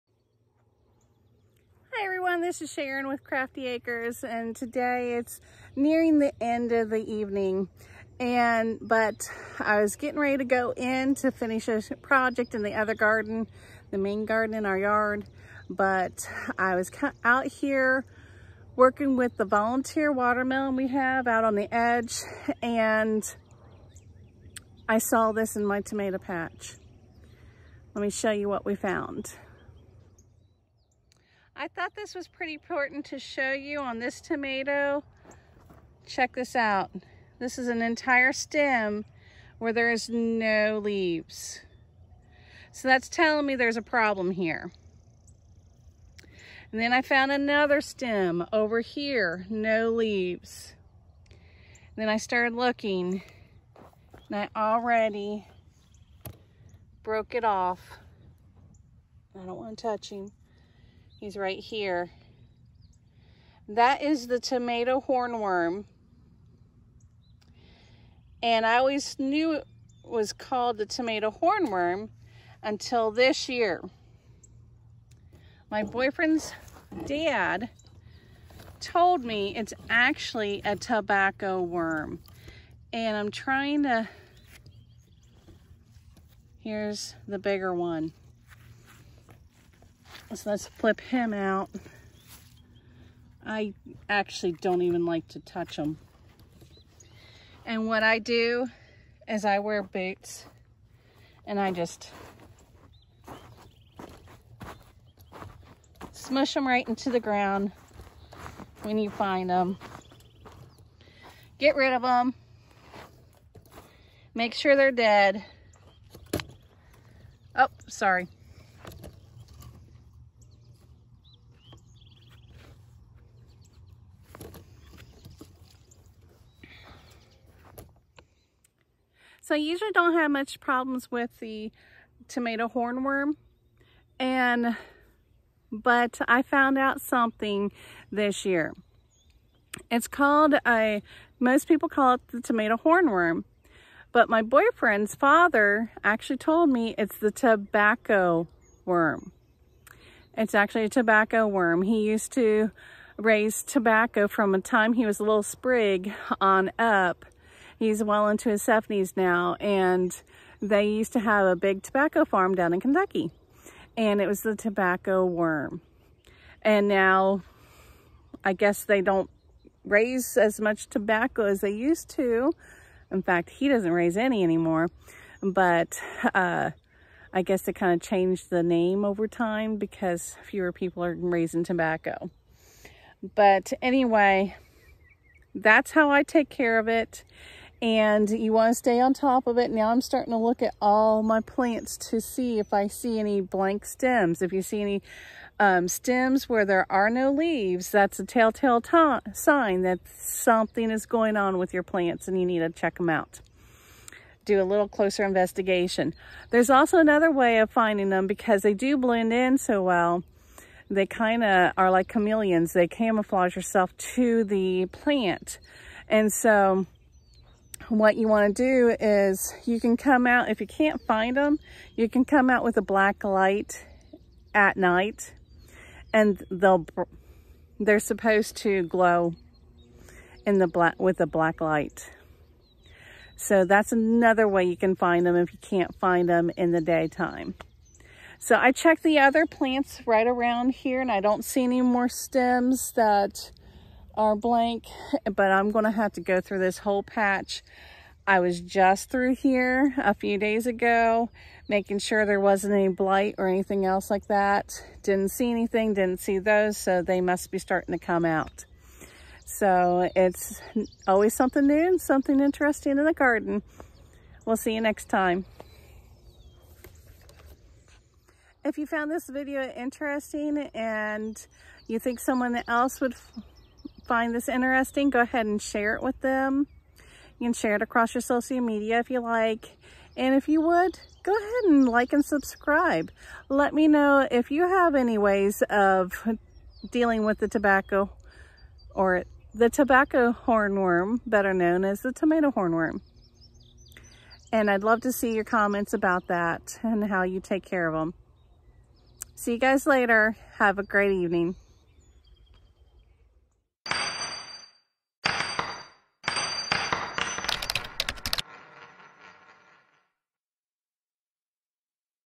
How to investigate problems in the tomato patch
Tomato horn worm, how to find and get rid of them.